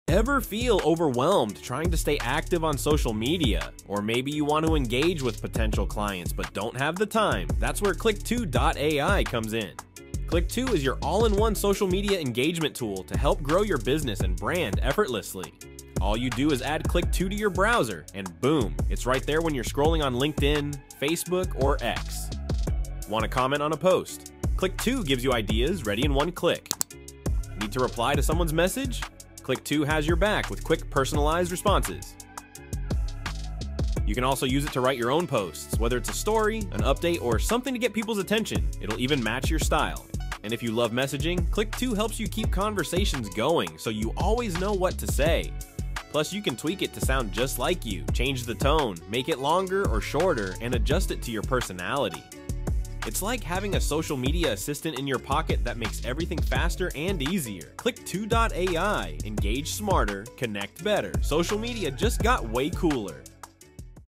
Effortlessly Boost Your LinkedIn/Fb/X Engagement with Click2.ai - AI Tool for Small Businesses
Discover how Click2.ai helps you skyrocket your LinkedIn/Facebook/X engagement while saving time! Leverage Click2.ai’s powerful features to automate connection building, boost post visibility, and increase sales—all while staying in control of your personal touch.

Whether you’re a small business owner or solopreneur, Click2.ai provides a simple, customizable way to amplify your LinkedIn presence and grow your business effectively.

💡 Features covered:

 • Effortless post generation
 • Direct messaging made easy
 • Smart LinkedIn contributions
 • Workgroup collaboration